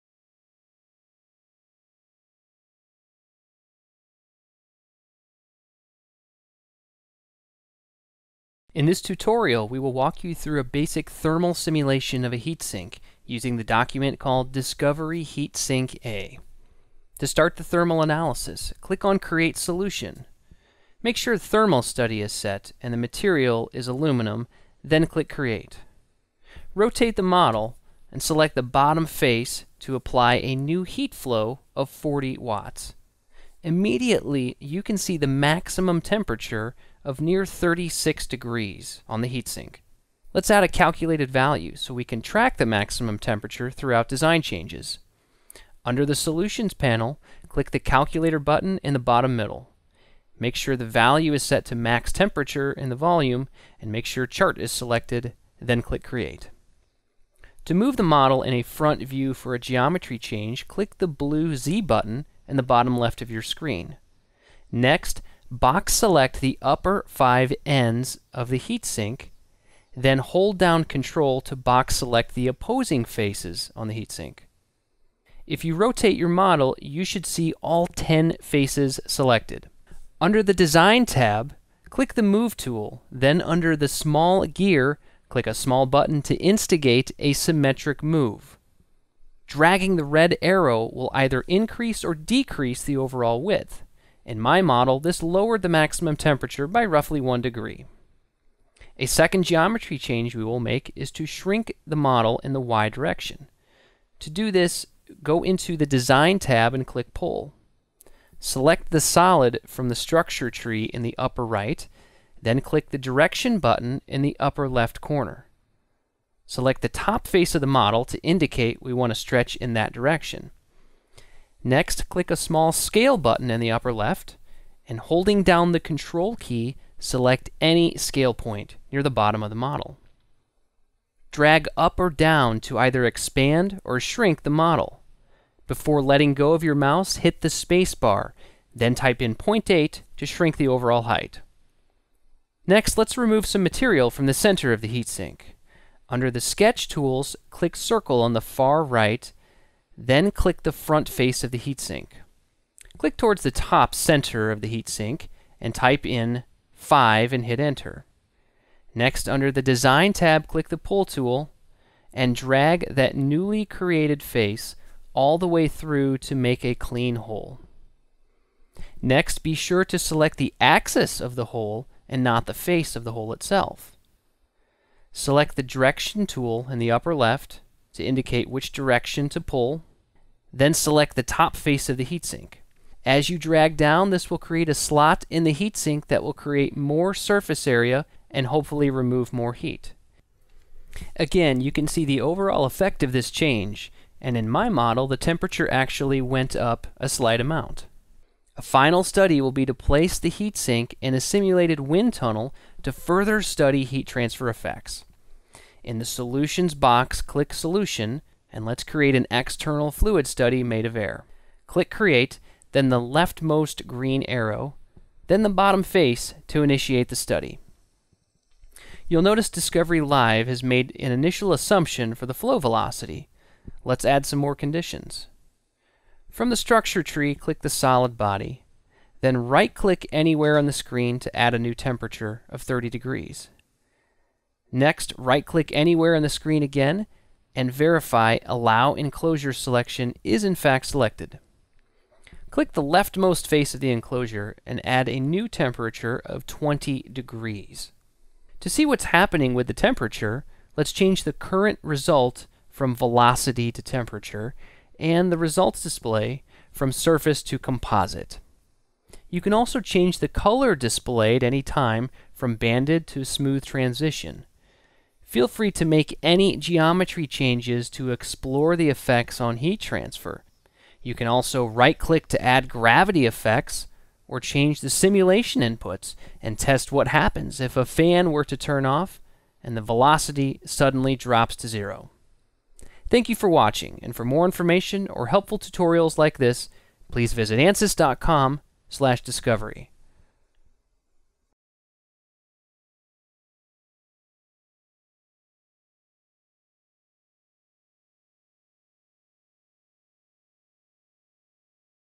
Discovery Live：熱解析のチュートリアル
このチュートリアルでは、「Discovery heat sink A」というドキュメントを使用し、ヒートシンクの基本的な熱シミュレーションについて、順を追って紹介していきます。